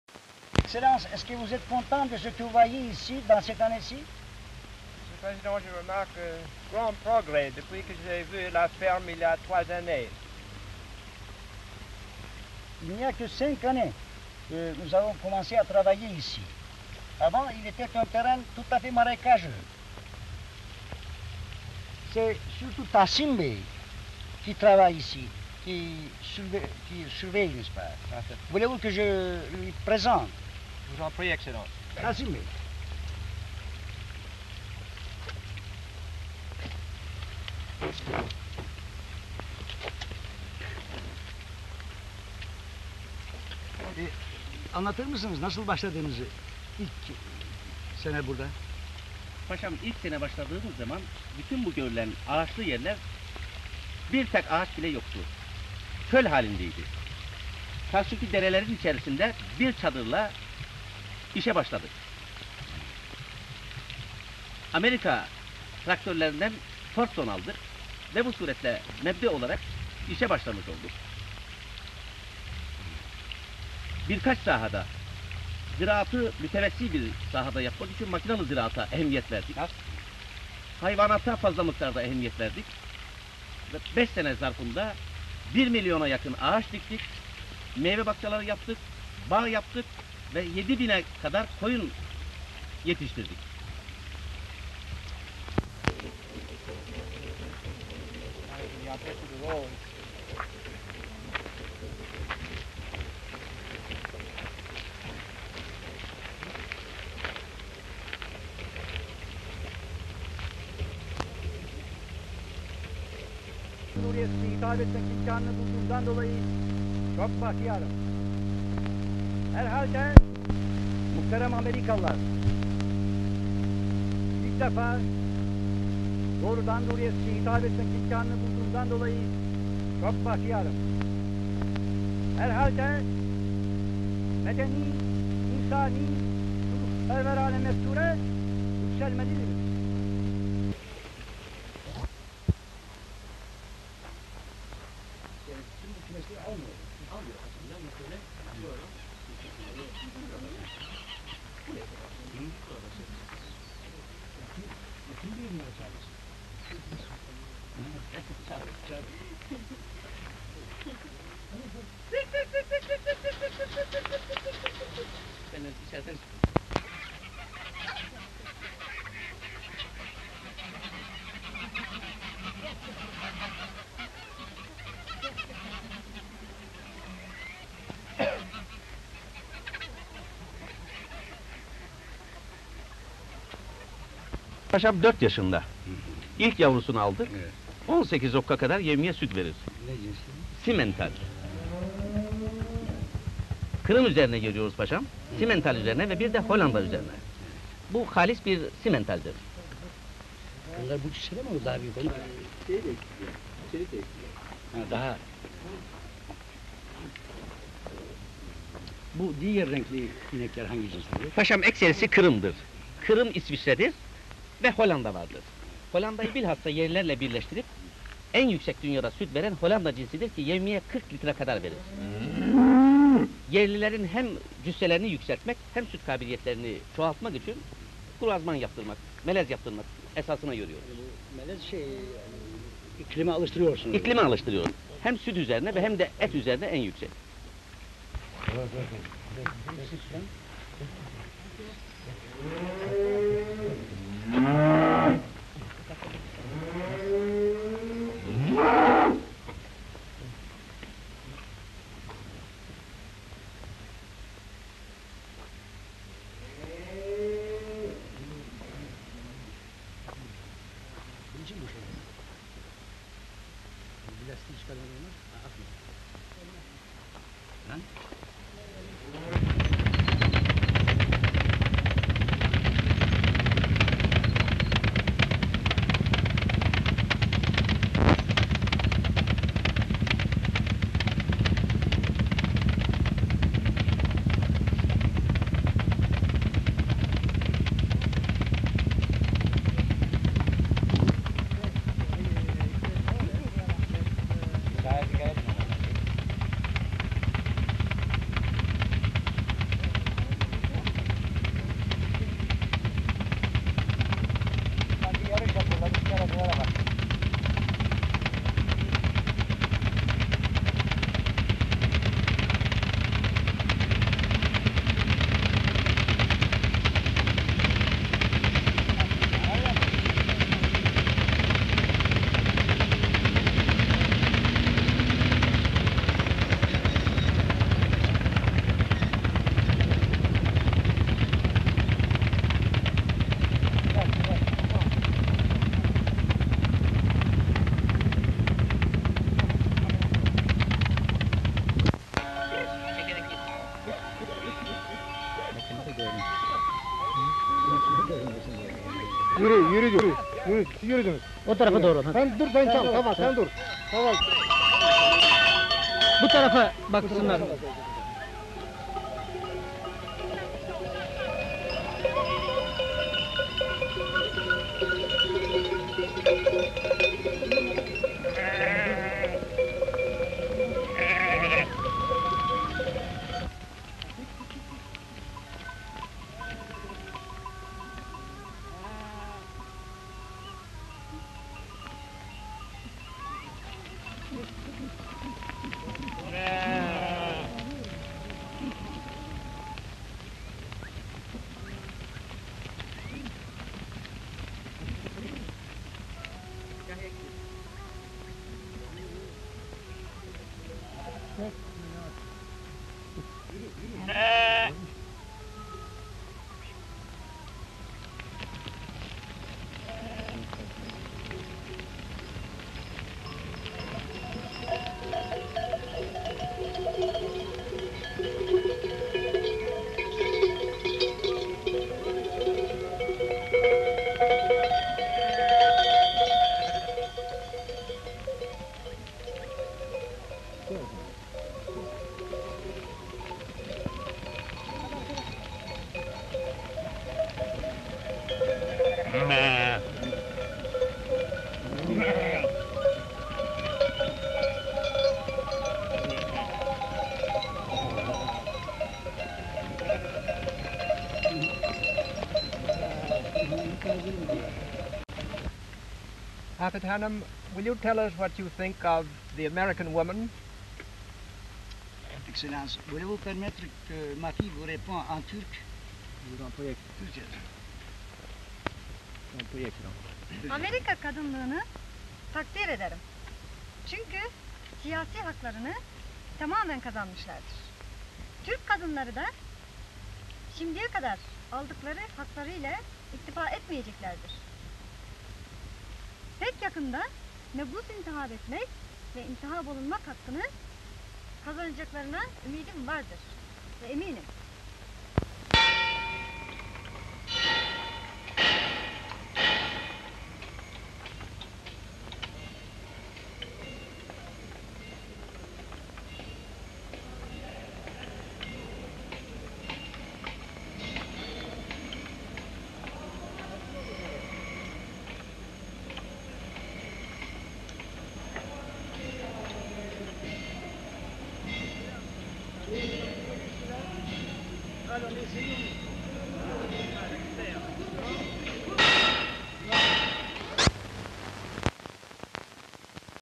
1930 - Atatürk Ve Afet İnan Atatürk Orman Çiftliği'nde
Daha önce parça parça yayınladığım Amerikan Büyükelçisi Joseph Grew'in Atatürk Orman Çiftliği ziyareti ve Kemal Atatürk ile Afet İnan'ın iyi niyet ve dostane diplomatik mesajlarını barındıran görüntülerinin uzun versiyonu.

İyi seyirler.